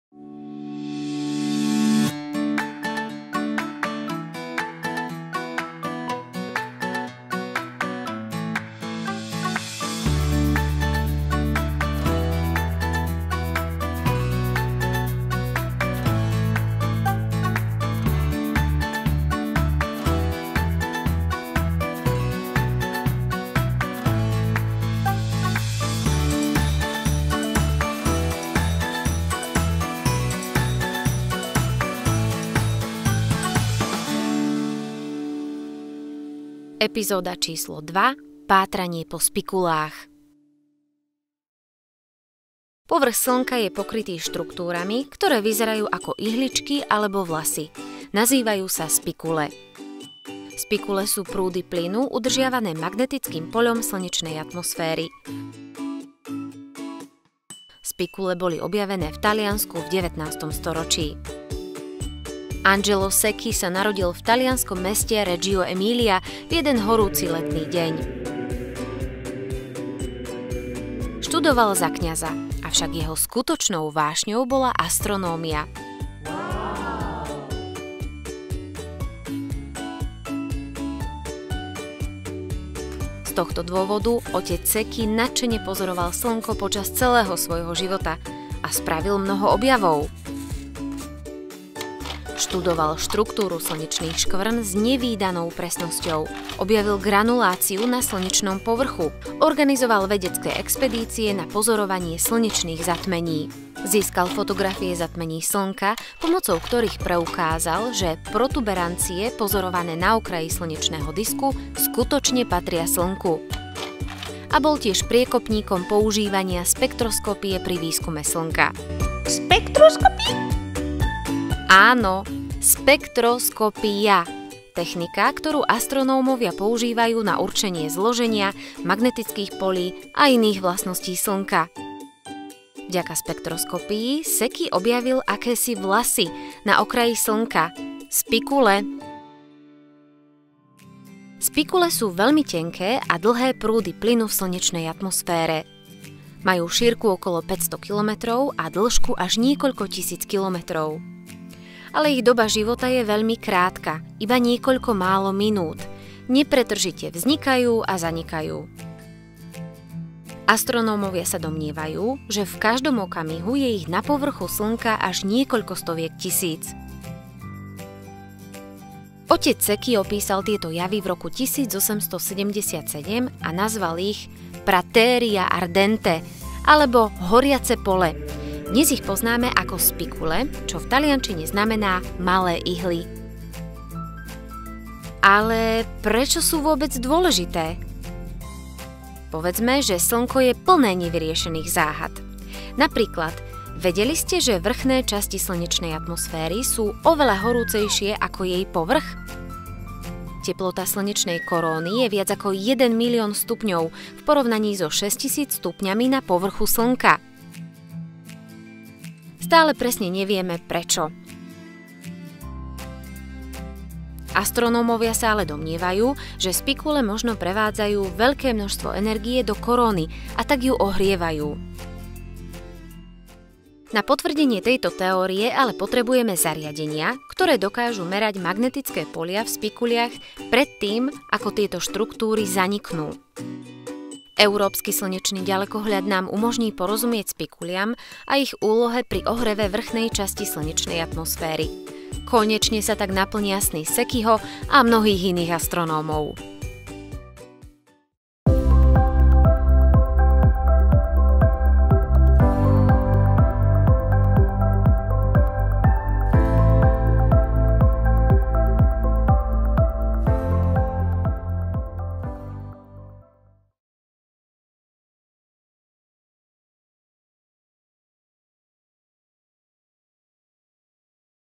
Epizóda II: The QuEST. Pátranie po spikulách
Spikule sú prúdy plynu udržiavané magnetickým poľom slnečnej atmosféry. Spikule boli objavené v Taliansku v 19. storočí. Slovenské rozprávanie: Monika Hucaková. Slovenská akadémia vied.

Nahovorila:
Monika Hucáková (Slovak Academy of Sciences)

Preklad:
Peter Zelina a Peter Gömöry (Astronomical Institute of the Slovak Academy of Sciences)

Strith a spracovanie zvuku:
Martin Bystriansky (Slovak Academy of Sciences)

Technická podpora:
Róbert Sabo (Institute of informatics of the Sloval Academy of Sciences)